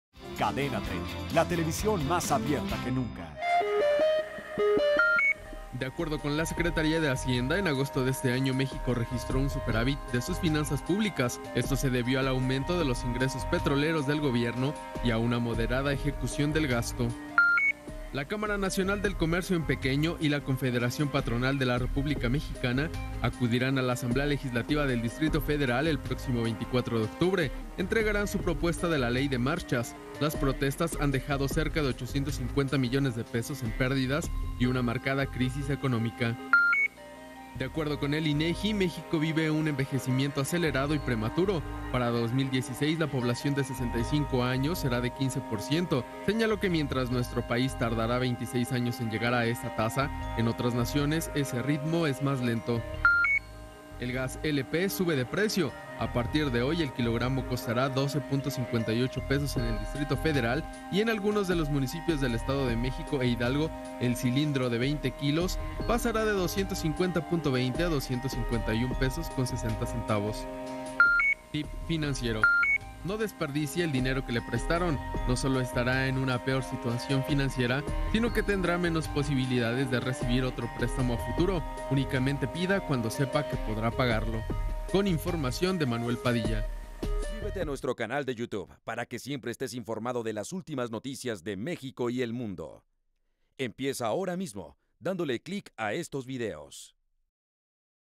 México registra superávit en sus finanzas públicas debido a los buenos ingresos petroleros
México registra superávit en sus finanzas públicas debido a los buenos ingresos petroleros/ tips financiero / Finanzas

1 de Octubre 2013

Esto se registró a principios del 2013. Día a Día te mostramos las finanzas más importantes de México y el mundo.